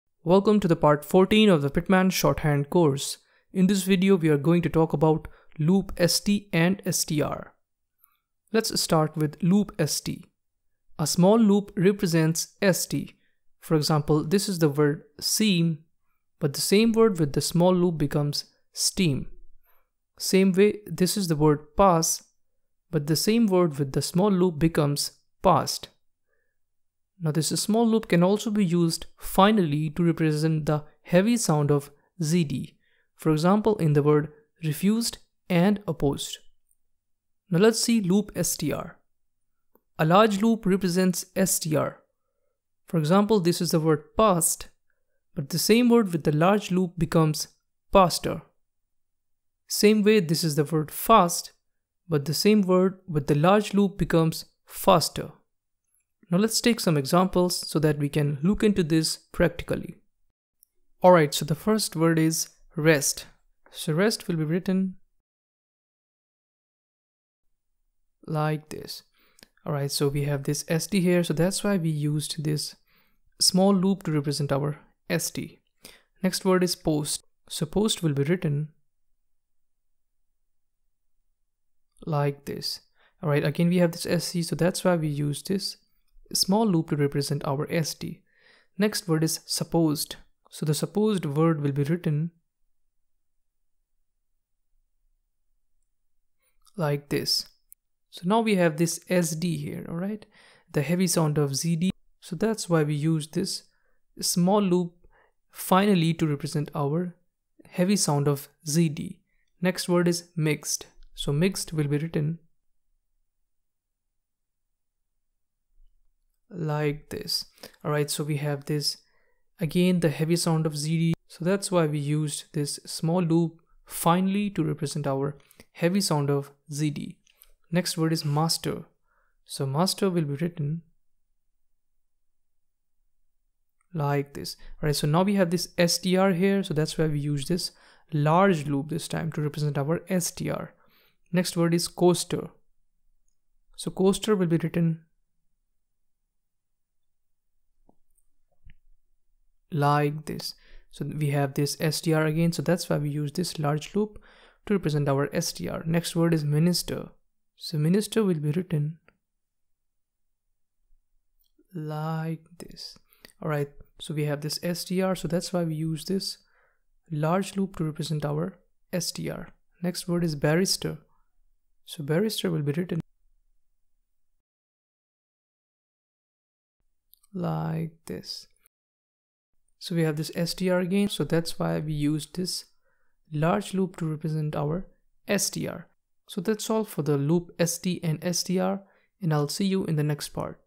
Pitman Shorthand Course (Part 14) Loop St & Str
This is PART 14 of the "Pitman" Shorthand course, in this video we are going to have a look at Loop St & Str.
(with Clear Audio & Video Quality) (Highly Recommended !!)

(without clear Audio & Video qualiity)